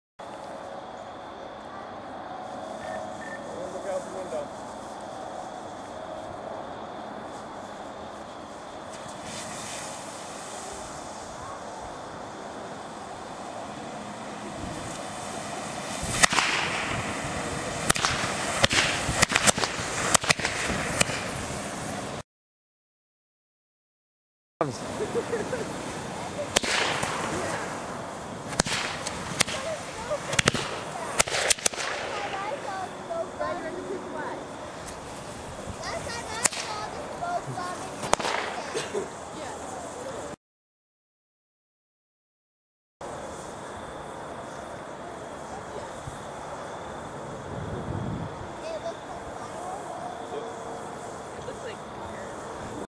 bad smoke devices
these where 75 winda brand Monmouth smoke devices that where pulled off the shelf because of nozzle problems the would propel themselves and/or blow up.